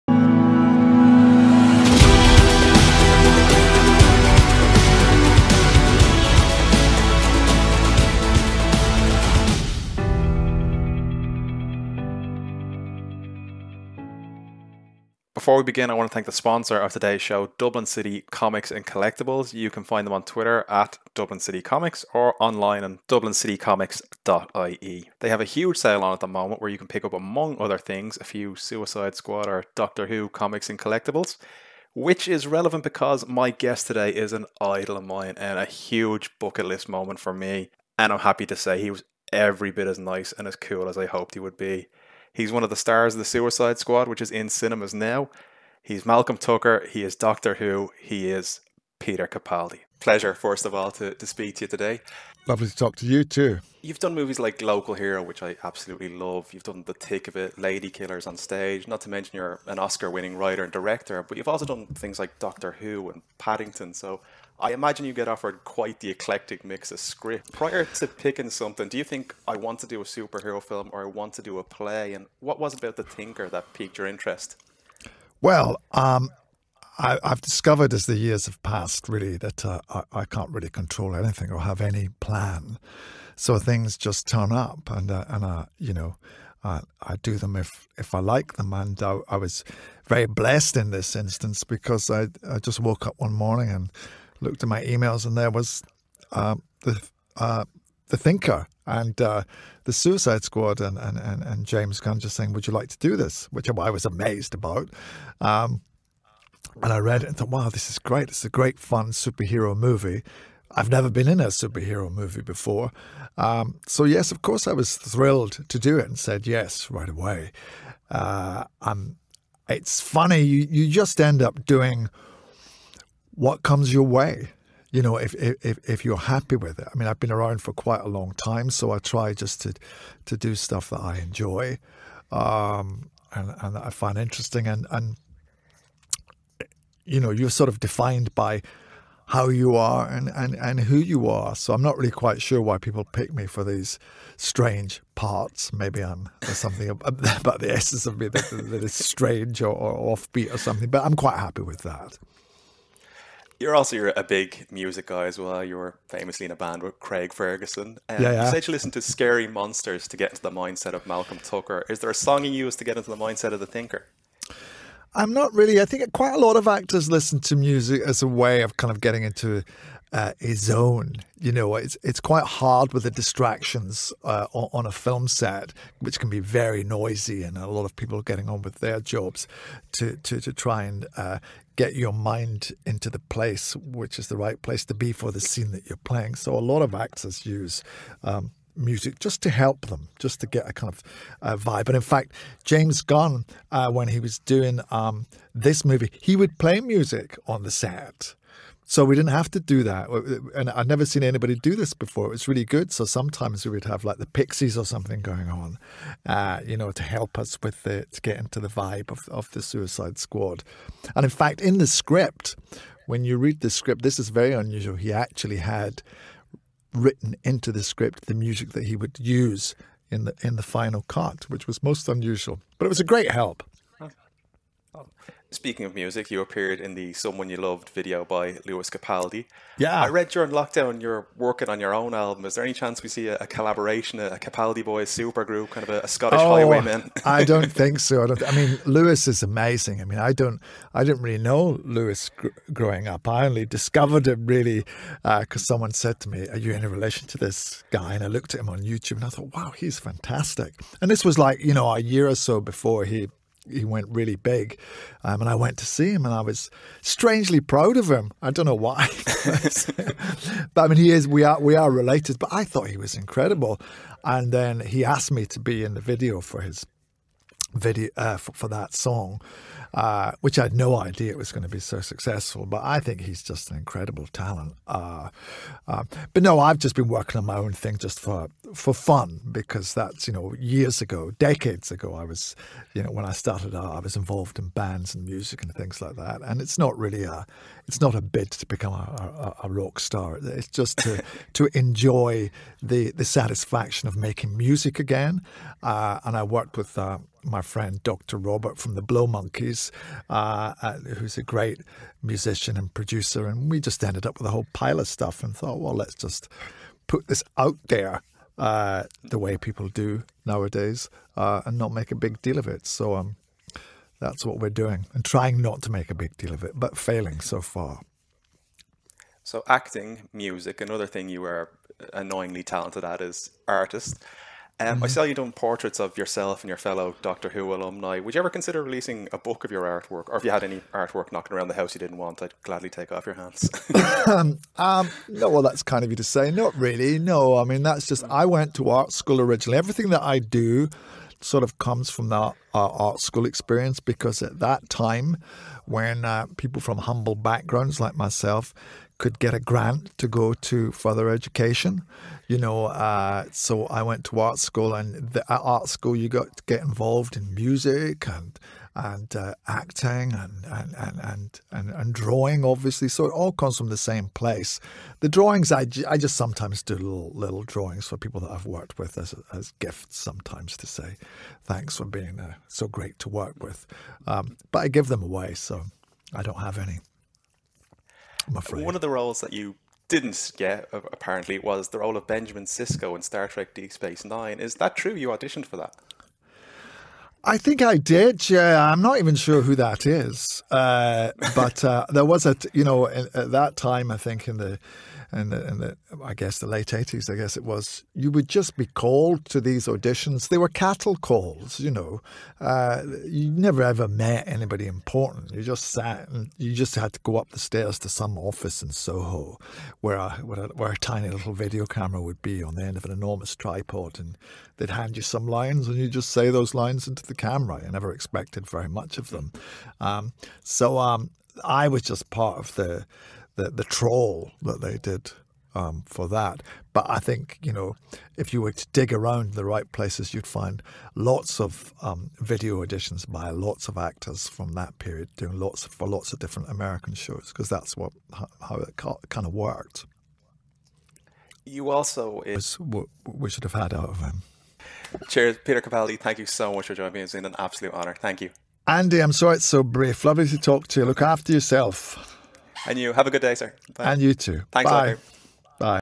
Peter Capaldi talks The Suicide Squad, Doctor Who, Star Trek, Lewis Capaldi & James Gandolfini.
Peter Capaldi talks about The Suicide Squad, Doctor Who, auditioning for Star Trek, Lewis Capaldi and working with James Gandolfini.